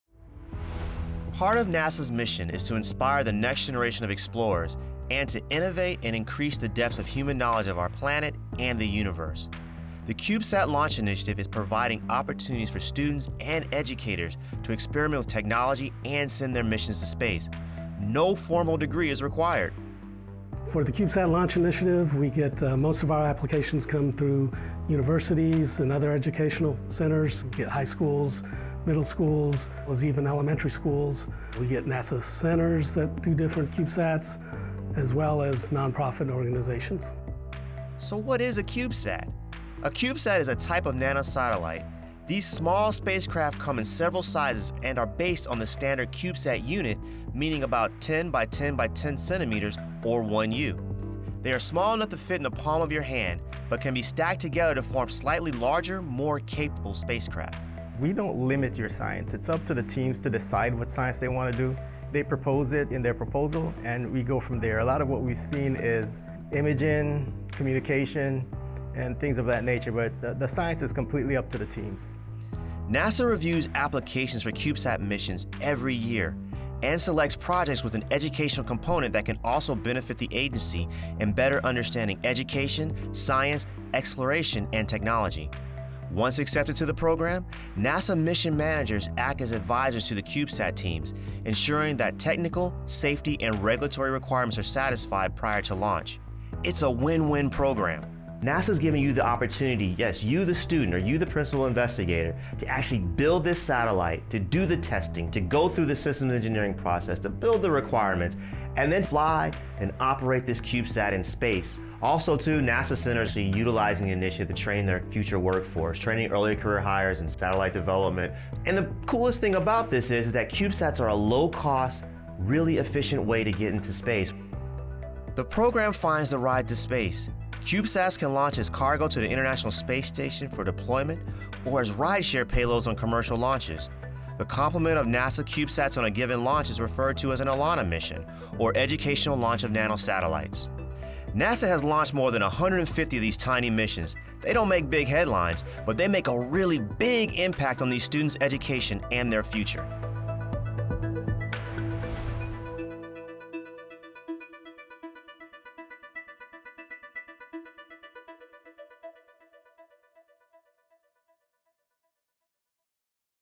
NASA Gives Students and Educators a Ride to Space
NASA’s CubeSat Launch Initiative provides opportunities for CubeSats built by U.S. educational institutions and non-profit organizations, including informal educational institutions such as museums and science centers, to fly on upcoming launches. There are new opportunities each year to apply to send CubeSats to space!

Credit: NASA

Follow NASA's Kennedy Space Center!